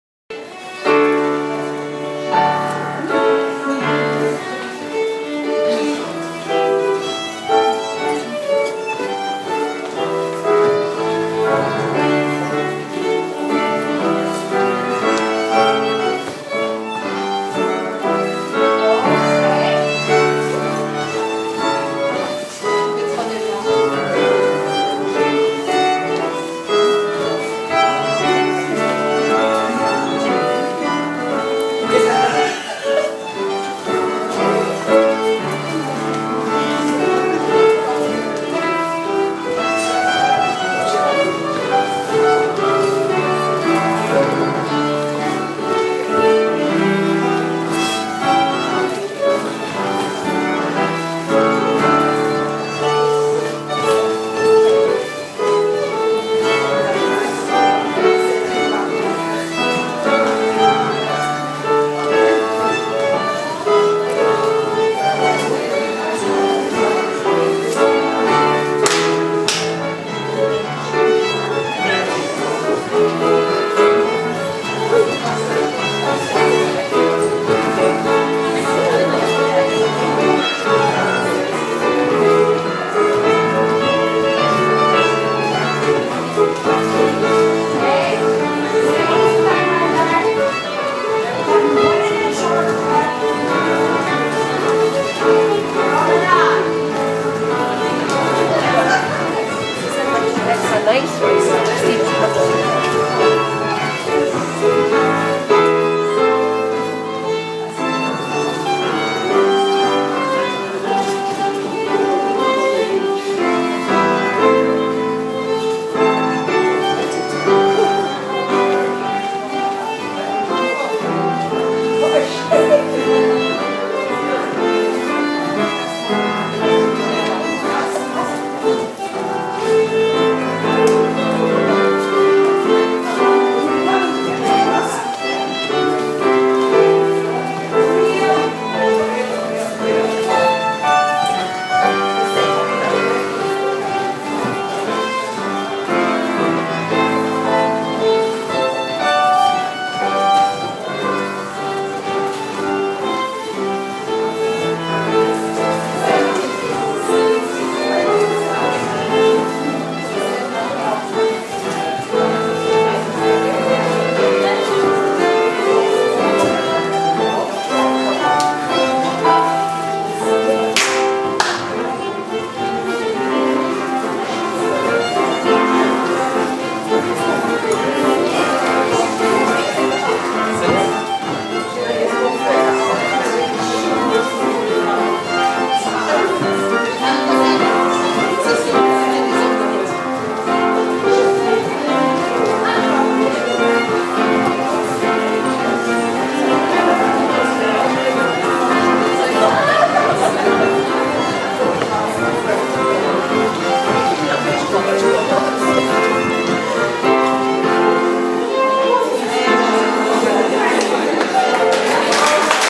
Rose of Glamis - 4c sq. S32+R32 - dev. by John Drewry (1990)
Recorded on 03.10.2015 during the RSCDS paris Branch weekend.